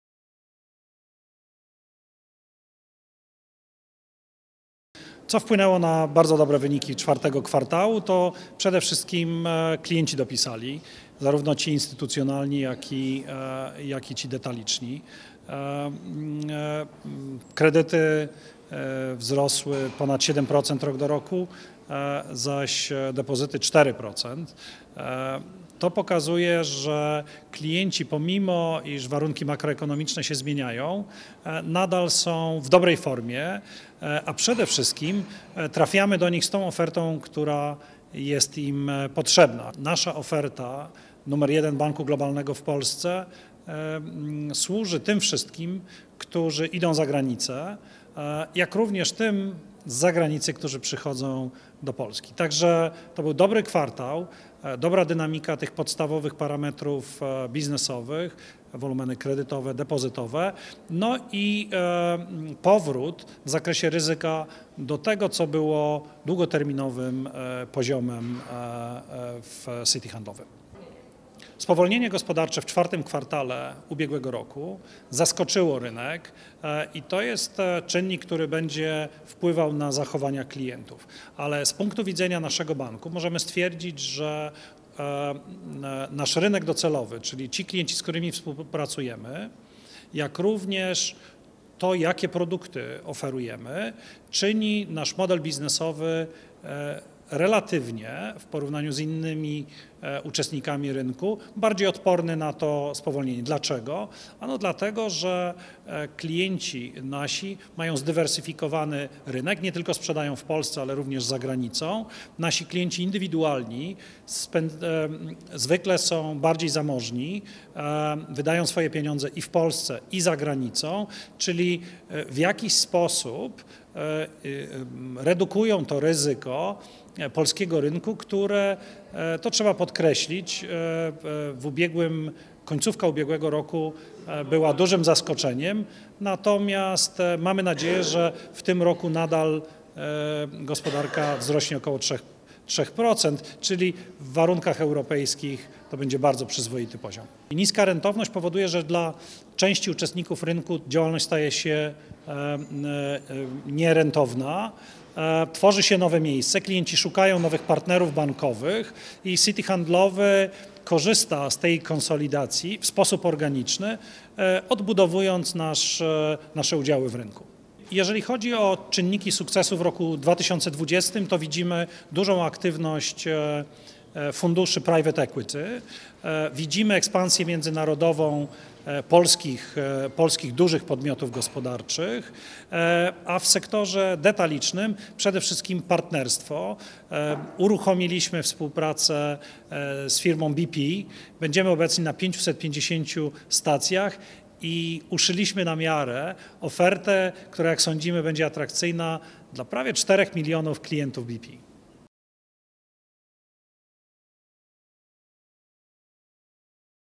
Sławomir S. Sikora, prezes Citi Handlowy
Na temat wyników Citi Handlowego oraz planów na 2020 rok rozmawialiśmy z prezesem banku Sławomirem S. Sikorą.

W ostatnim kwartale 2019 roku Citi Handlowy utrzymał stabilną dynamikę wzrostu przychodów, na co największy wpływ miała działalność skarbcowa. W połączeniu ze ścisłą kontrolą kosztów i utrzymaniem kosztów ryzyka na niskim poziomie, pozwoliło to bankowi zanotować najlepszy czwarty kwartał od pięciu lat.